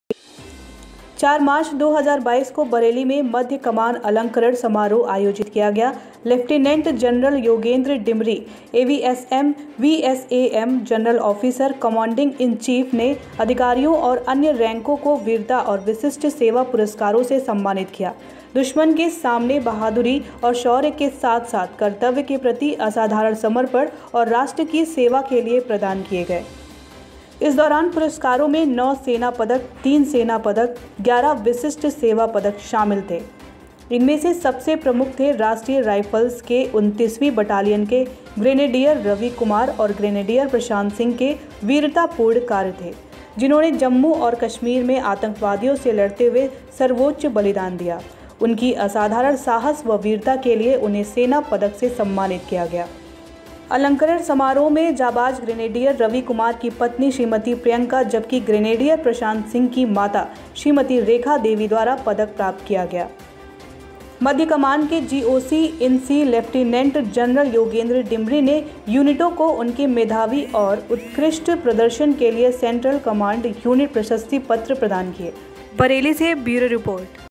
मध्य कमान अलंकरण समारोह बरेली में आयोजित किया गया।
4 मार्च, 2022 को बरेली में मध्य कमान अलंकरण समारोह आयोजित किया गया । लेफ्टिनेंट जनरल योगेंद्र डिमरी, एवीएसएम, वीएसएम, जनरल ऑफिसर कमांडिंग-इन-चीफ (जीओसी-इन-सी) ने अधिकारियों और अन्य रैंकों को वीरता और विशिष्ट सेवा पुरस्कारों से सम्मानित किया। दुश्मन के सामने बहादुरी और शौर्य के साथ-साथ कर्तव्य के प्रति असाधारण समर्पण और राष्ट्र की सेवा के लिए प्रदान किए गए।